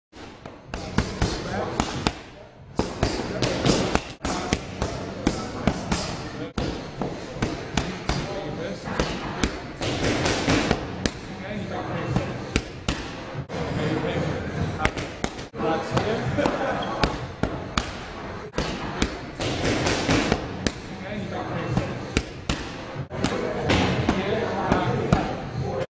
Focus Mitts - Boxing Training - Technique
Who wants to learn Coach Nelson (Supreme Team Boxing) Pad Work?
Here I start out fast but then we slow it down that way you guy can practice at home.